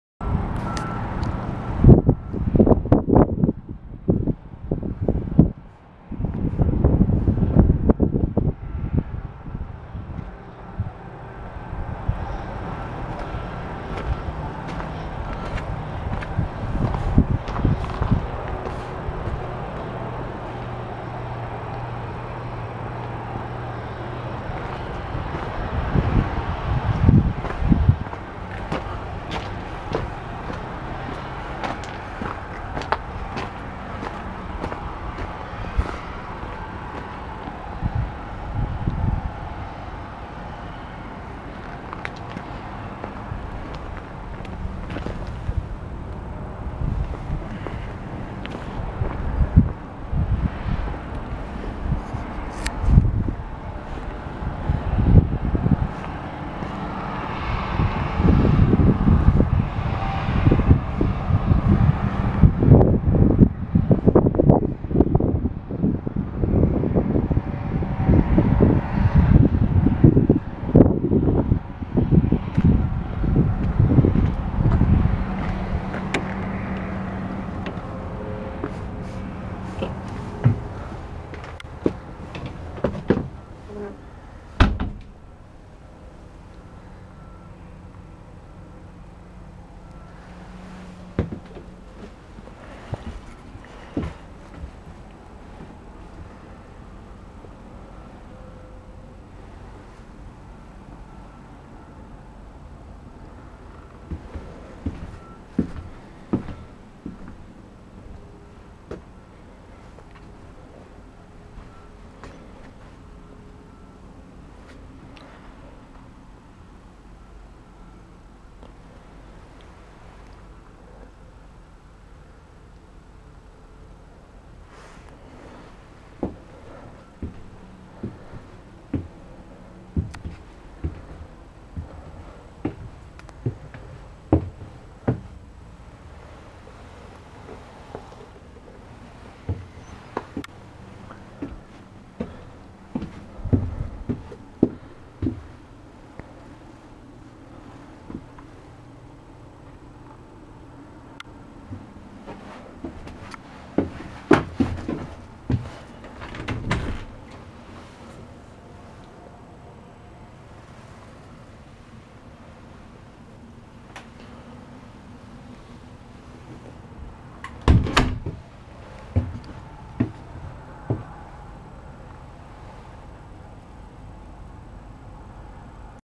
2009 SUNSEEKER 2860 DS | Olds RV | Calgary, Airdrie, Red Deer, Sundre, Carstairs, Edmonton
A NICE CLEAN FOREST RIVER 2 SLIDE CLASS C MOTORHOME, GENERATOR, AND ONLY 42,000 KMS ON IT, 2009 CHASSIS, 2010 CLASS C RV, JUST ARRIVED !!